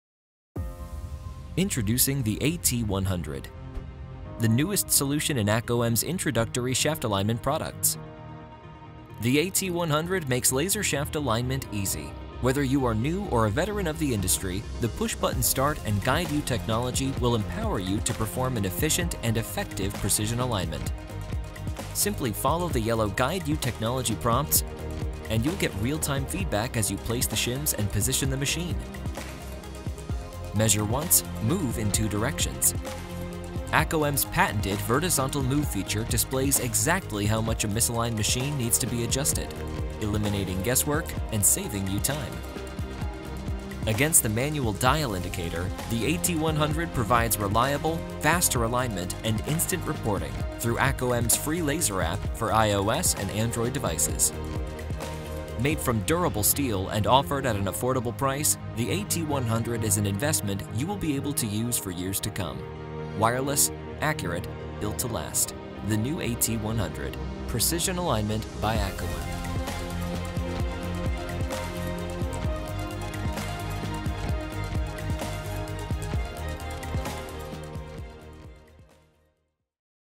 ACOEM AT-100, entry-level shaft alignment tool.
• Fast and accurate results are delivered by
intelligent sensor technology.
• Step-in shaft alignment that is easy to use and affordable at the same time.
• Reports in PDF format show the alignment process and results in detail. Reports can be easily shared.
• Live animations of the engine's position make the measurement and horizontal  alignment intuitive and easy.